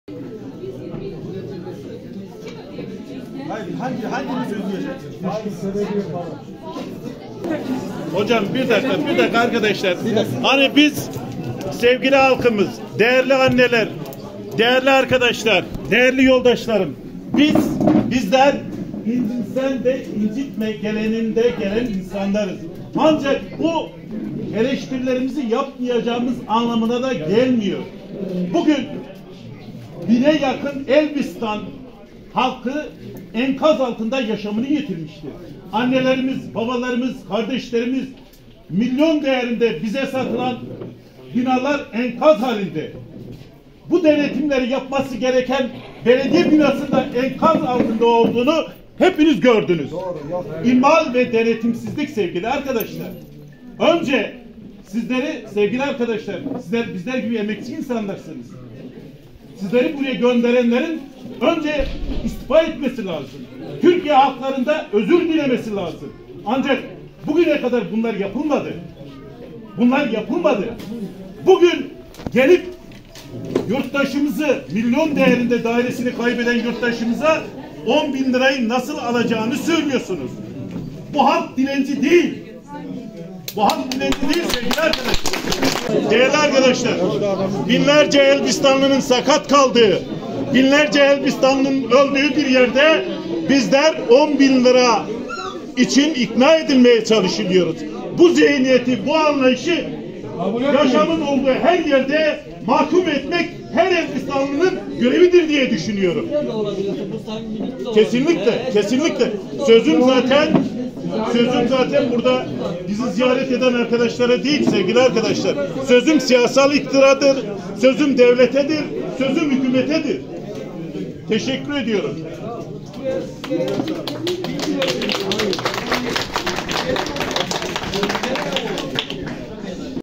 DEPREMZEDELERDEN AFAD’A TEPKİ: SİZİ GÖNDERENLER İSTİFA ETMELİ
Elbistan’da bulunan Cemevine gelen AFAD ekiplerine tepki gösteren depremzedeler, “Sizleri buraya gönderenlerin istifa etmesi, özür dilemesi gerekiyor” dedi.

Depremin merkez üssü olan Mereş’in Elbistan ilçesinde bulunan Hacı Bektaş Veli Anadolu Kültür Vakfı (Cemevi) gelerek iktidarın depremzedelere vereceğini açıkladığı maddi yardıma (10 bin TL) ilişkin bilgi vermek isteyen AFAD yetkililerine halk tepki gösterdi. Depremzedeler, evlerinin depremde yıkıldığını ve bu yıkımlardan devletin sorumlu olduğunu söyledi.

‘BU HALK DİLENCİ DEĞİL’

Deprem nedeniyle evleri yıkılan depremzedeler için yapılacak konutların yüzde 40’ının yurttaşlar tarafından ödeneceğini söyleyen AFAD ekiplerine tepki gösteren bir yurttaş, “Sizleri buraya gönderenlerin istifa etmesi, özür dilemesi gerekiyor. Bugüne kadar bunlar yapılmadı. Milyon lira değerindeki dairesini kaybeden yurttaşımıza 10 bin TL’yi vermeyi nasıl söyleyebiliyorsunuz? Bu halk dilenci değil” ifadelerini kullandı.

Tepkiler üzerine AFAD ekipleri Cemevi’nden ayrıldı.